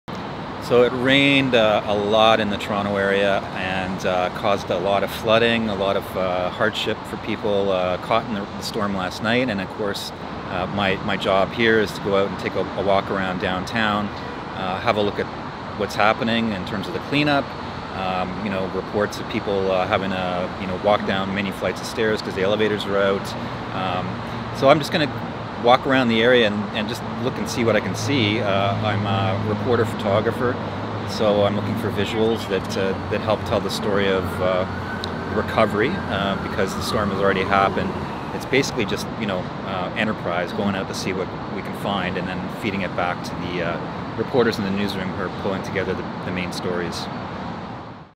Jim Rankin on covering Toronto's flood
The Star's Jim Rankin explains his role in covering the flash flooding that hit the inner core of Toronto.